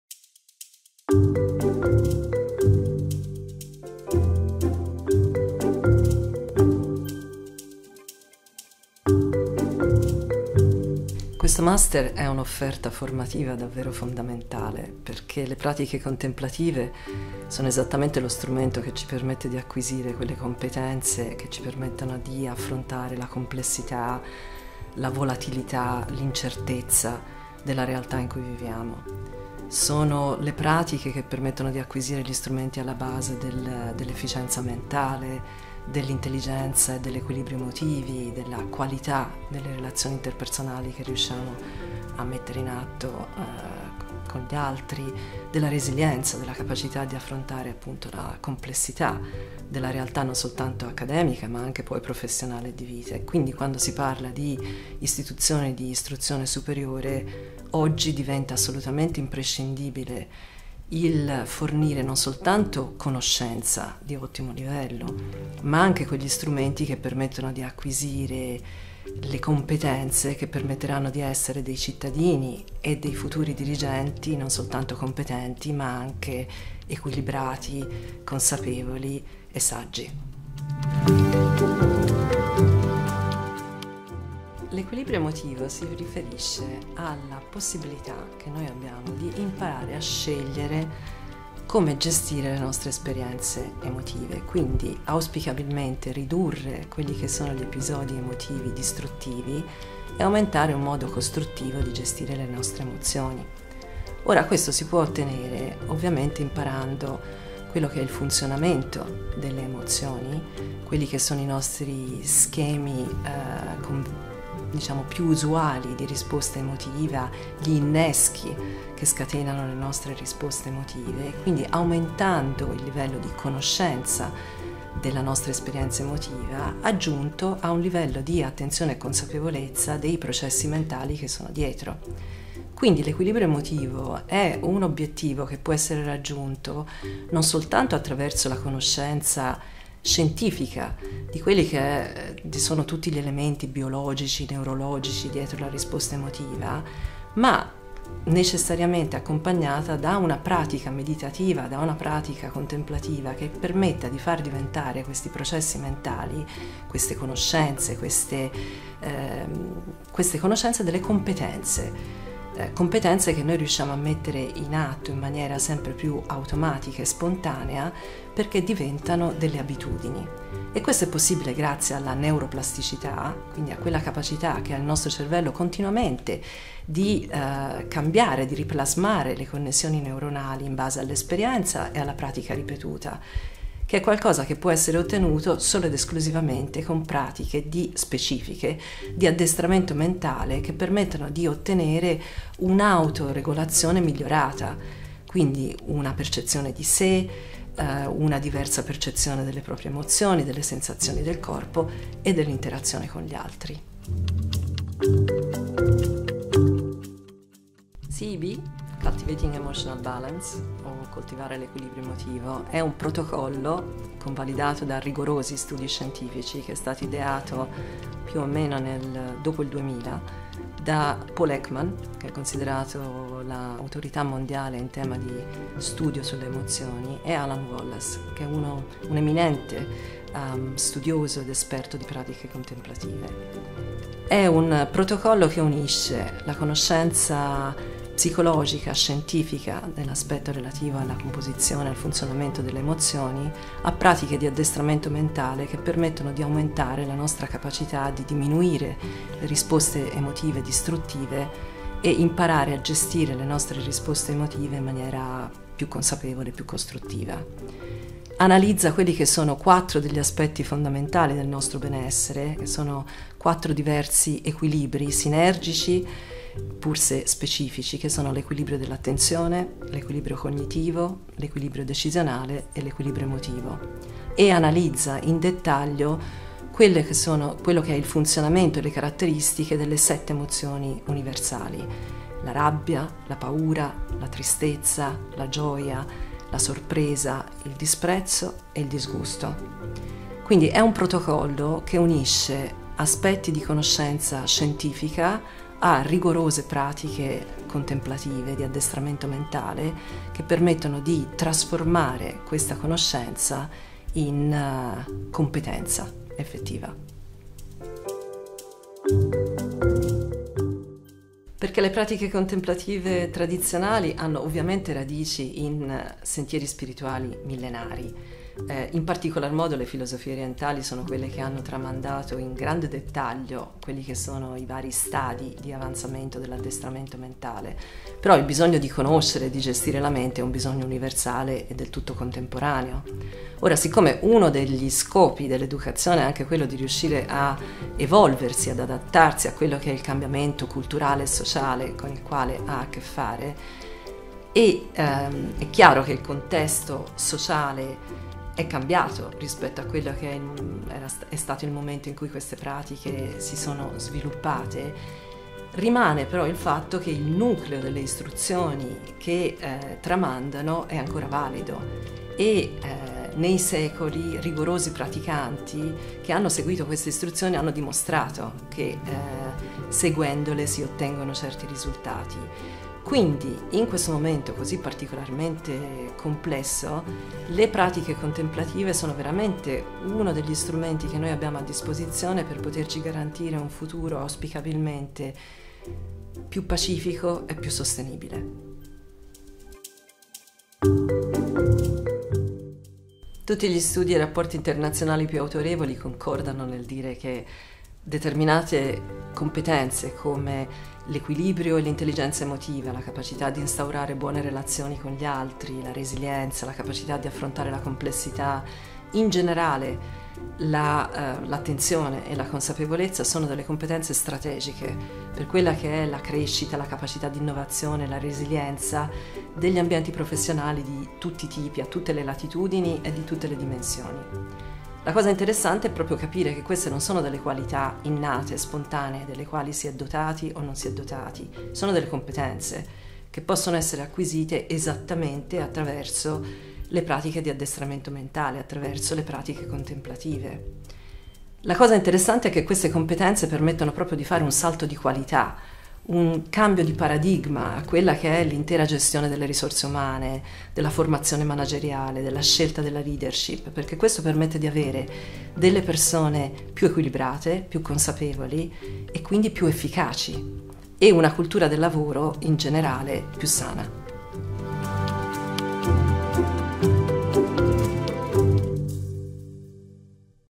Valentina Dolara parla del Master in “Contemplative Studies”
Valentina Dolara – insegnante, formatrice e co-fondatrice del Mind of Joy Consulting – parla del Master in “Contemplative Studies: la pratica della meditazione dalle tradizioni al contemporaneo”, nel cui contesto conduce il training meditativo “Cultivating Emotional Balance” (CEB).